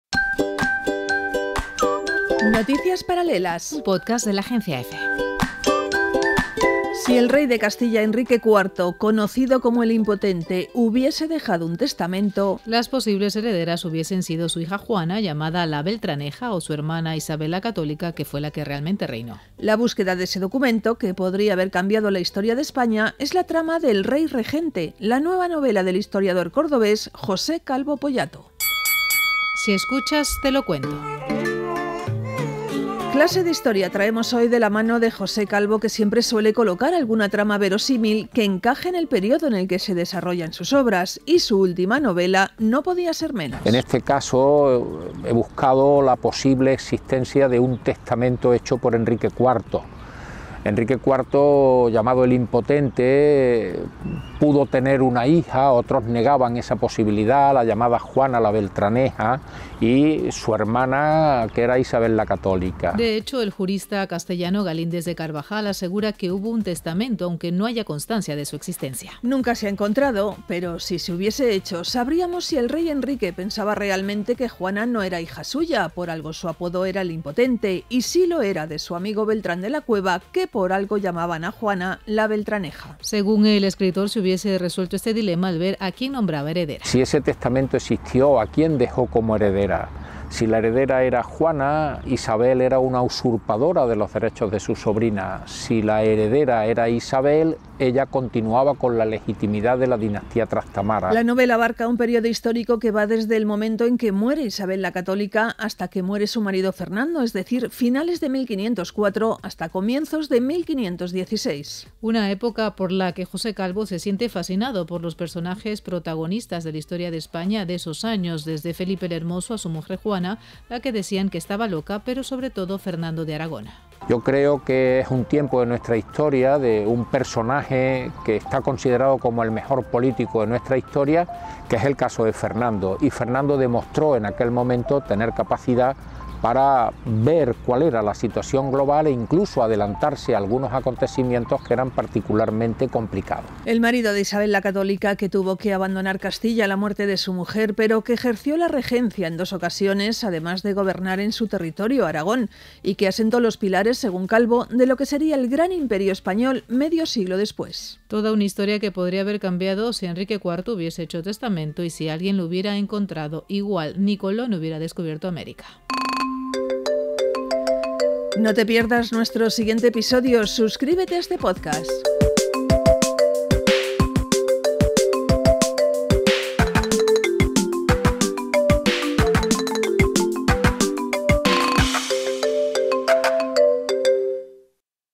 El testamento que pudo cambiar España | Noticias Paralelas
Si el rey de Castilla Enrique IV, conocido como 'el impotente', hubiese dejado un testamento las posibles herederas hubiesen sido su hija Juana, llamada 'la Beltraneja', o su hermana, Isabel, la Católica, que fue la que realmente reinó. La búsqueda de ese documento, que podría haber cambiado la historia de España, es la trama de 'El rey regente', la nueva novela del historiador cordobés José Calvo Poyato. Si escuchas te lo cuento...